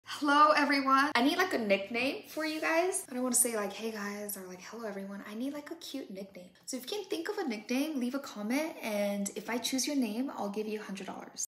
Help me to pick a name and I'll give you $100!
My channel @SarahTyau is about transforming clothes but also other areas in life! Tell me what I should name our community and if I choose your name as the winner, I will give you $100 cash.